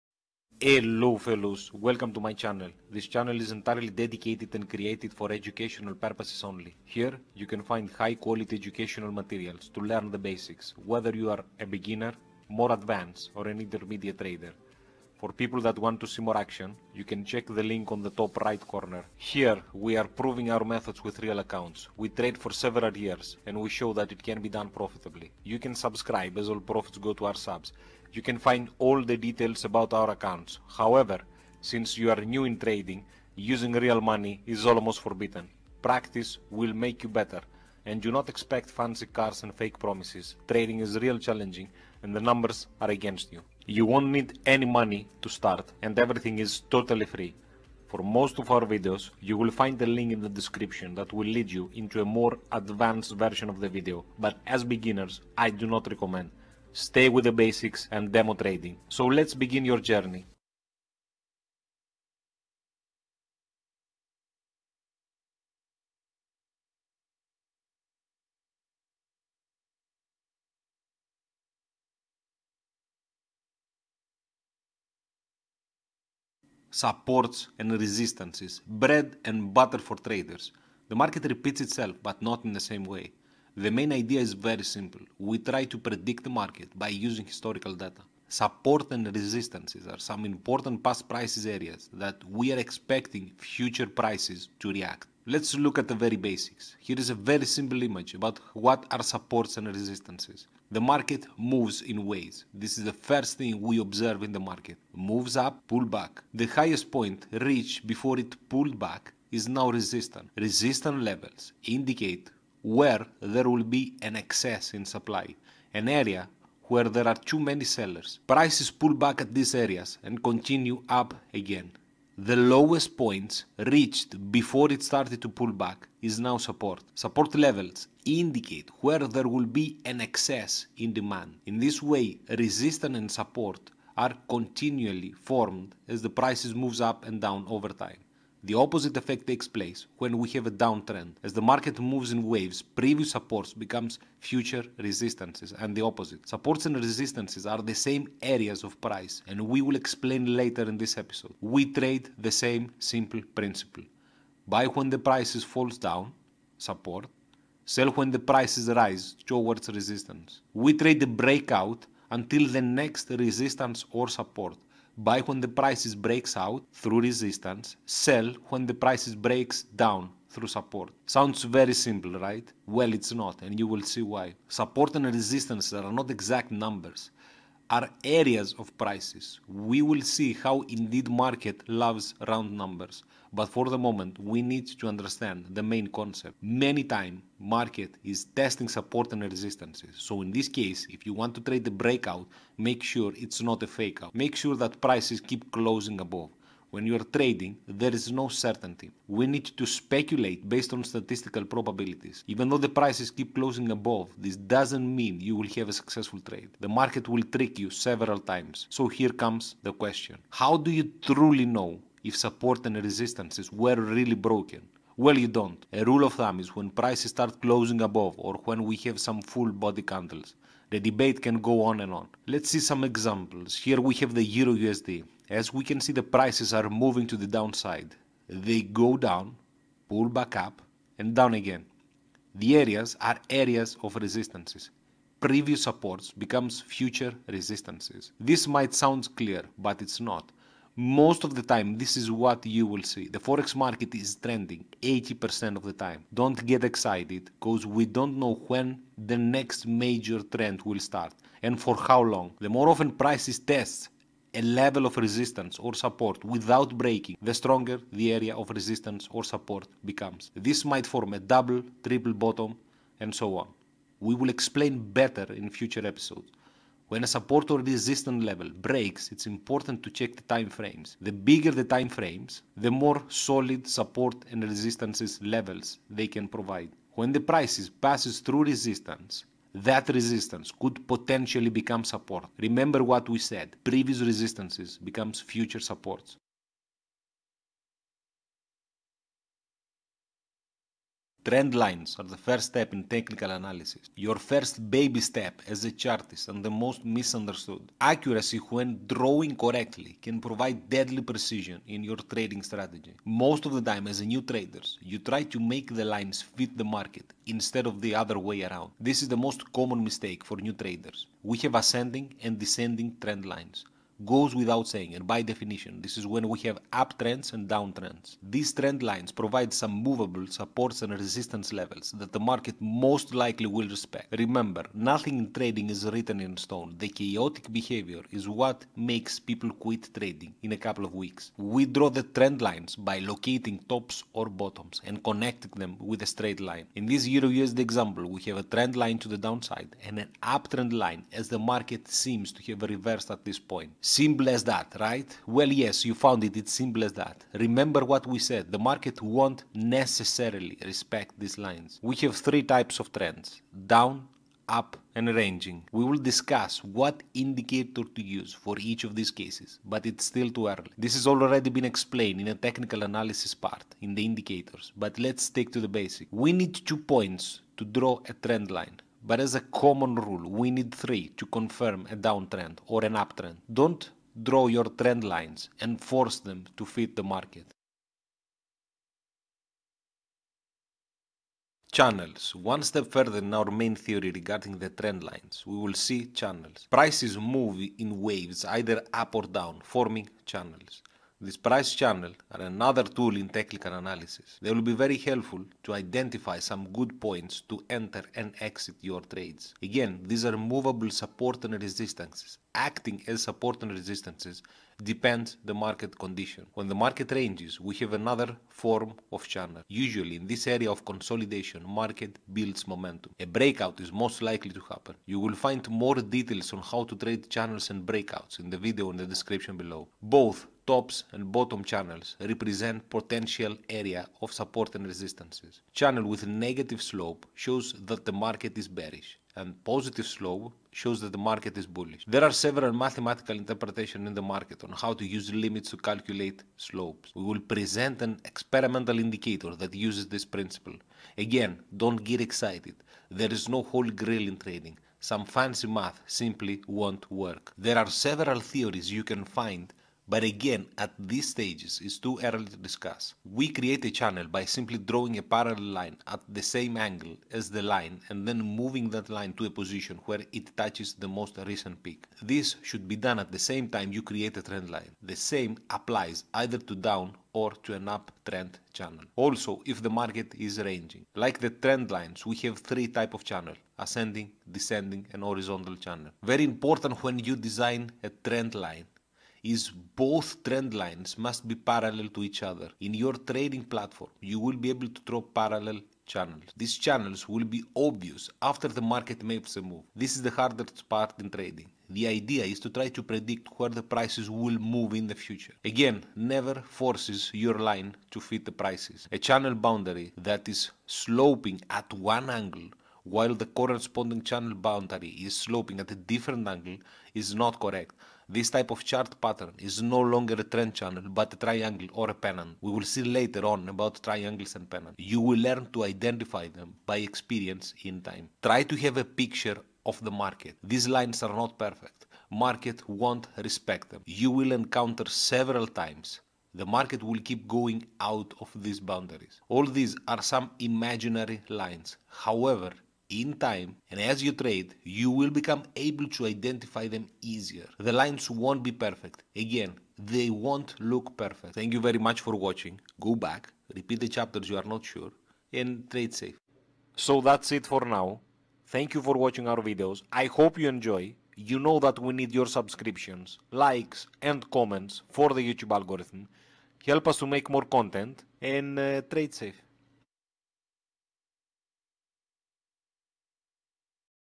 Trading supports and resistances, for beginners. Lesson 10/50.(learn how to trade)
Hello, fellows,
This is the 10th episode of the series. Supports and resistance are been explained in this episode.
Trend lines and channels. How they work and how, as a beginner, you can easily identify them. In this episode, we are analyzing the trend lines and how they can help you in your trading technique. The idea is simple however the market won't always have a clear direction. Most of the time as traders we need to identify and make a trading decision.
As beginners, it won't be easy to identify the trend lines only after the market makes the move.

Contents of this video:
00:00 Intro
01:10 1)Supports and resistances
04:48 2) Trend lines
06:18 3) Channels

useful links :
useful links :
I hope you enjoy it!!!
Again trade safe fellows !!